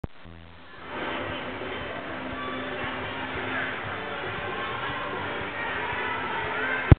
Split Closing 180- Top View- Synchronized Swimming
Watch how the left side needs to move forward around the circle as well as the left elbow needs to pull forward in front of the body.  The left elbow should be in line with the right elbow right from the start.  It is already behind and plays catch up the entire twist.  Then into the vertical the right side comes all the way back to the left side instead of meeting in the middle.